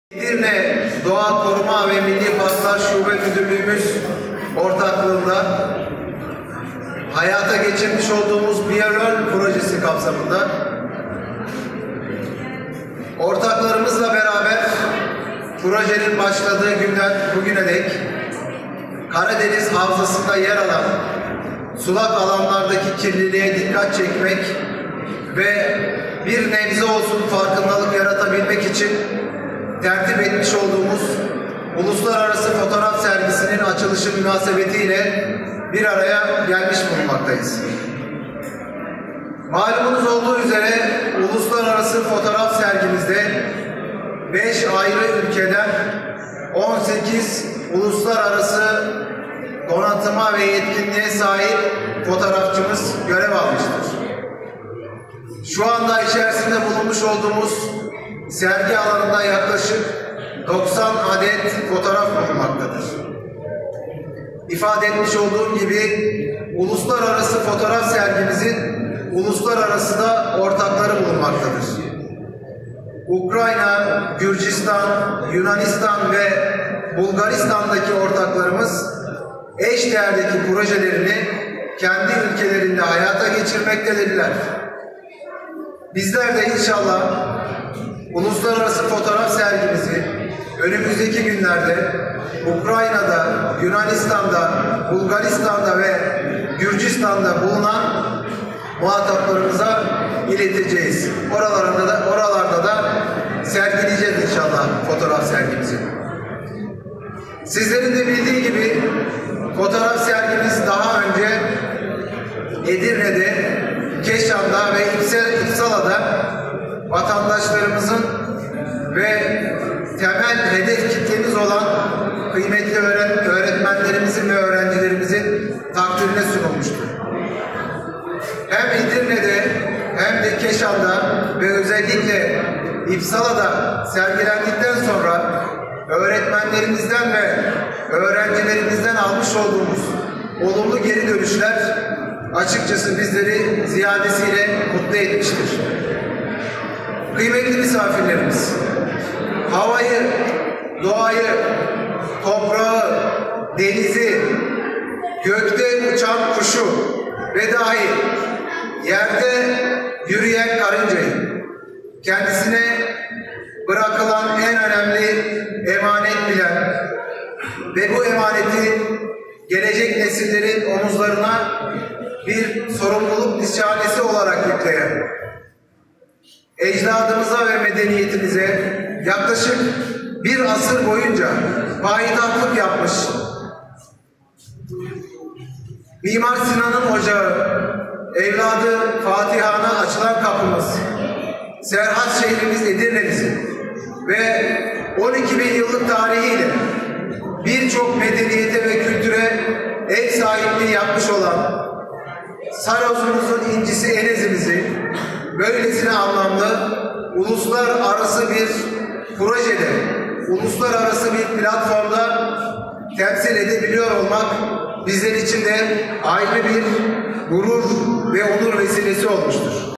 Enez'de uluslararası çevre fotoğrafları sergisi
Sergi, Bulgaristan,Yunanistan ve Ukrayna'da da açılacak...